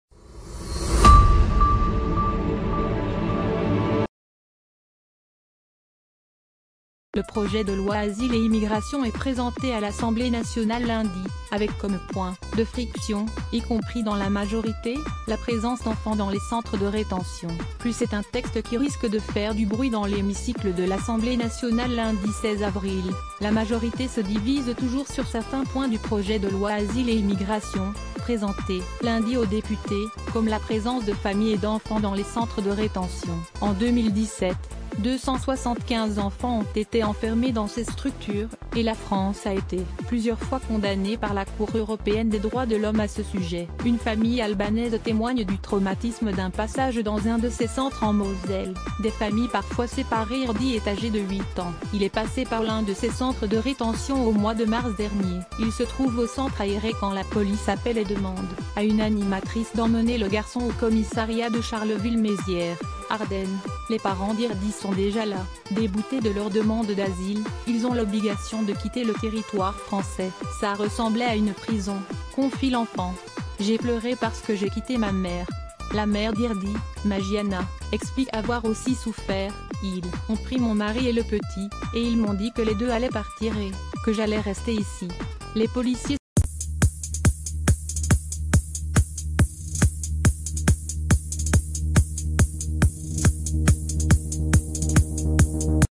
"Ça ressemblait à une prison", témoigne un enfant albanais, passé par un centre de rétention de Mose
"Ça ressemblait à une prison", témoigne un enfant albanais, passé par un centre de rétention de Moselle